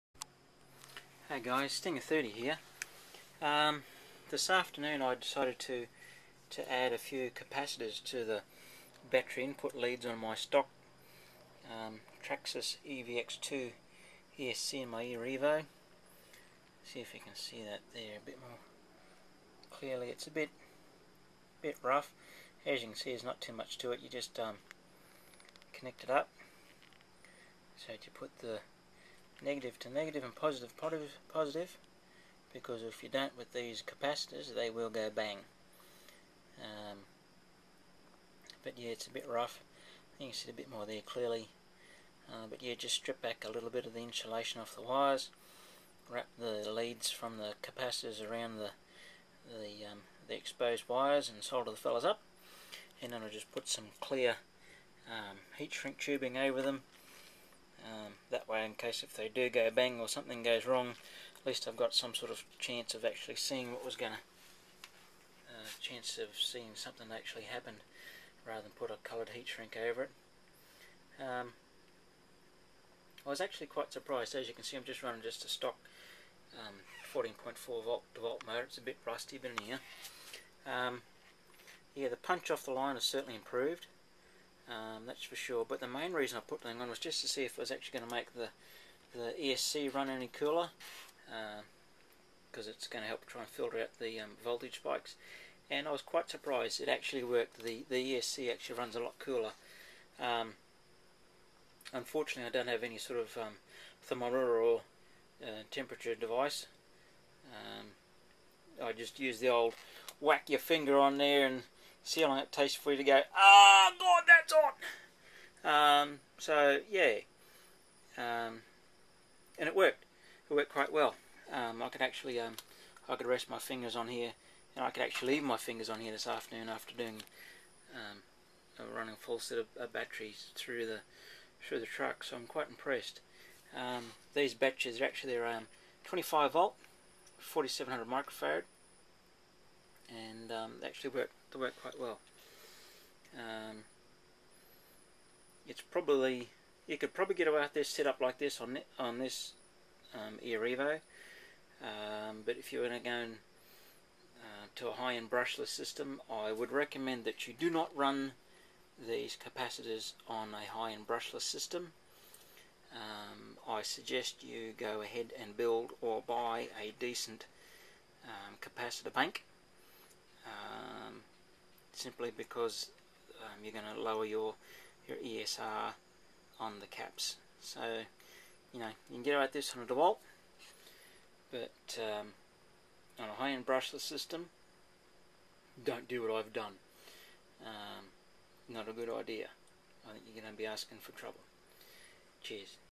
added capacitors to stock traxxas evx-2 esc
Capacitors on battery leads to esc

I have been reading for some many months about this idea and most of it I have read was from owners of Castle Creations MMM (Mamba Monster) esc on various forums on the net

seems to be a lot of MMM esc going boom but it seems to have been minimised by adding capacitor banks

Castle sell their own

you can also make your own as well

the general consensus seems to be that by adding caps it will lower the heat of the esc, take the ripple out of the voltage spikes and other good stuff

this document explains a number of issues

this document explains how to add capacitors to the battery input on an esc

this afternoon just for kicks I thought I would add 2 caps to my battery leads to my stock traxxas evx-2 esc, just for fun.one on each of the battery leads from the esc.
The caps I used are 4700uF 25Volt. I honestly thought it would make no difference as im not using a high-end brushless setup, just a boring old 14.4v dewalt drill motor.

Total investment of these 2 caps was just $7.50
Well im pleased to say it did make a difference. The esc certainly feels cooler to touch after a decent bashing session, I unfortunately don't have any kind of accurate temperature measuring device of any kind other then whack my finger on it and see how long it takes to be burnt to a crisp.

Any high grade Low ESR capacitor will work. Just match the capacitor voltage to your batteries.

6.6V Cap is for 4-Cell NiMH
10V Cap is for 4cell, 6Cell or 2S Lipo
16V 3S Lipo and lower voltage
25V 5S Lipo and lower voltage

Any capacitor in the 1500uf to 10,000uf range will work.

Incase your wondering, ESR = Equivalent Series Resistance

hope this helps someone, im no expert on this, just found it interesting enough to share